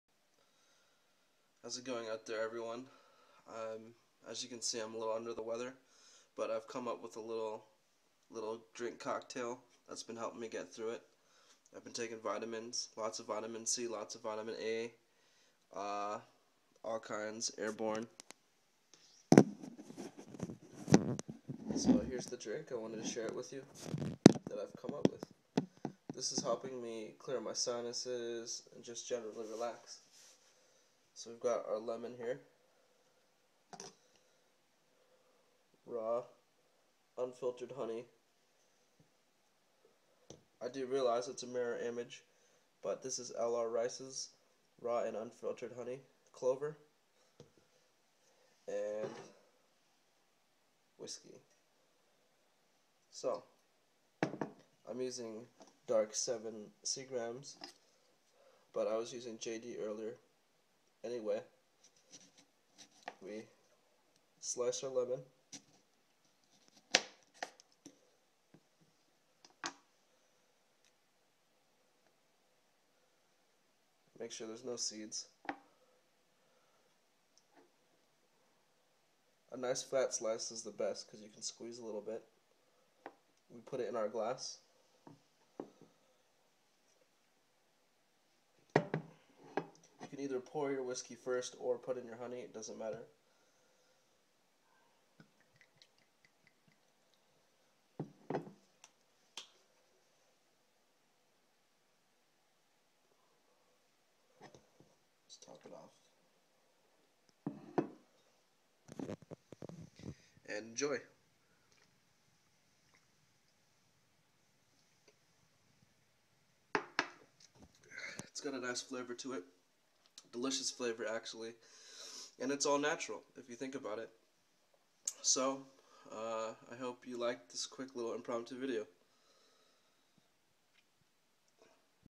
"The Flu Shot"
A drink i created. Never take the real one, take mine.